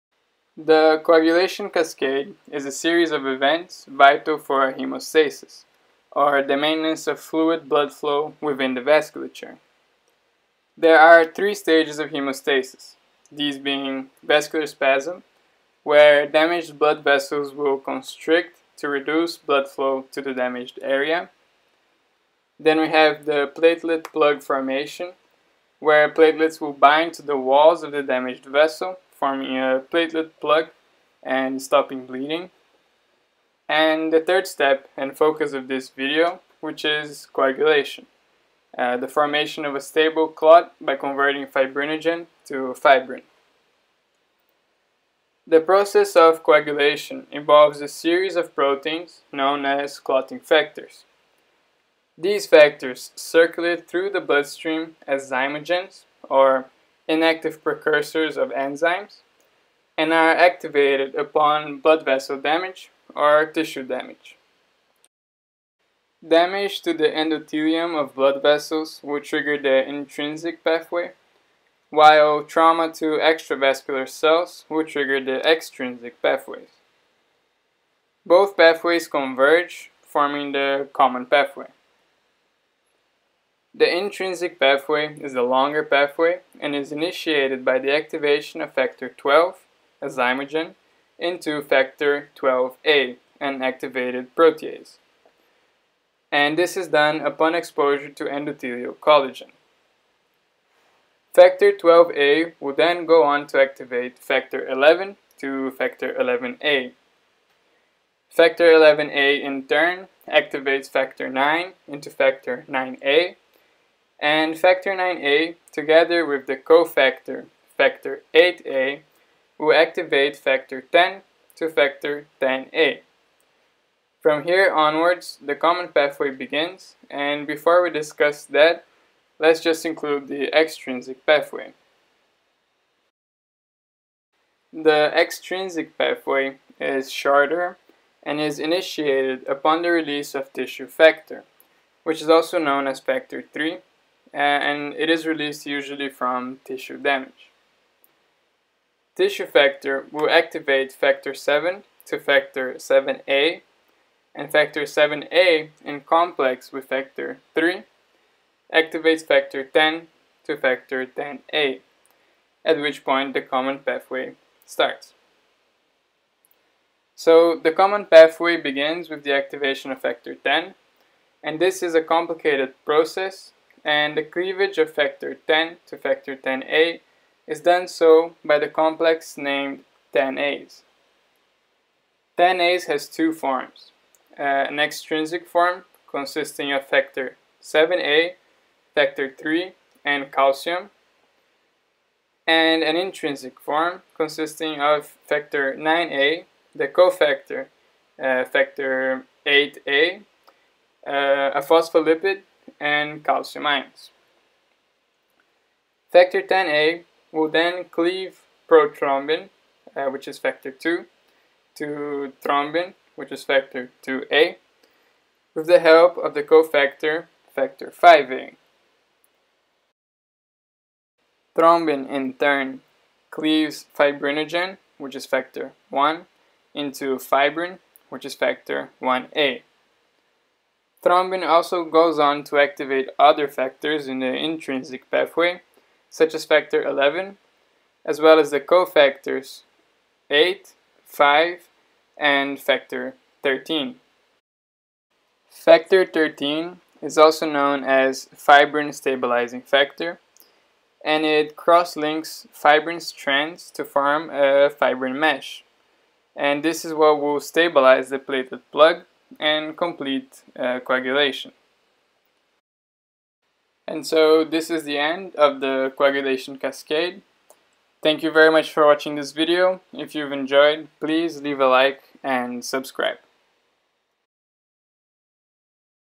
Coagulation cascade (simple!) [Haemostasis, Intrinsic pathway, Extrinsic pathway]
This video talks about the coagulation cascade, describing the intrinsic, extrinsic, and common pathways. The coagulation cascade comprises of the activation of clotting factors, which are zymogens (inactive precursors of enzymes).